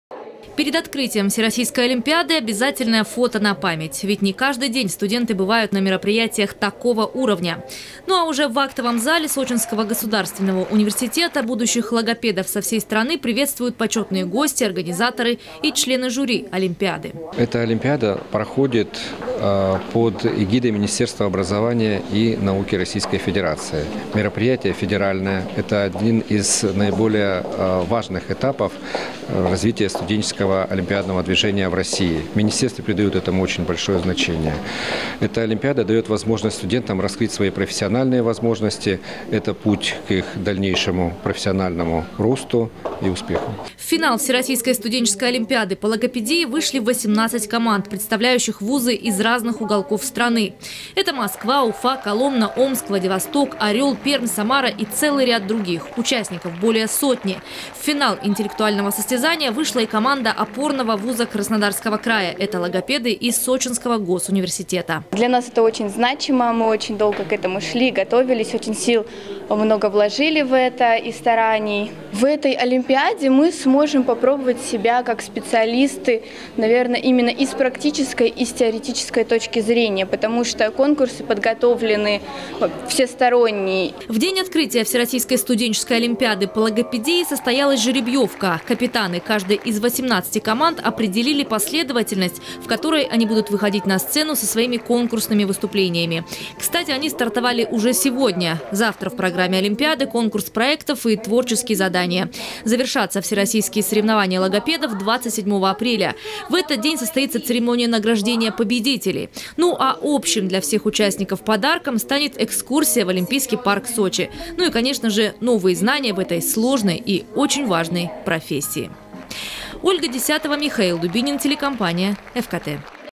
Cочи принимает Всероссийскую студенческую олимпиаду по логопедии. Новости Эфкате
Финальный её этап организовал и проводит Сочинский государственный университет — опорный ВУЗ Краснодарского края.

Сегодня состоялось торжественное открытие этого масштабного мероприятия. Будущих логопедов со всей страны приветствовали почётные гости, организаторы и члены жюри Олимпиады.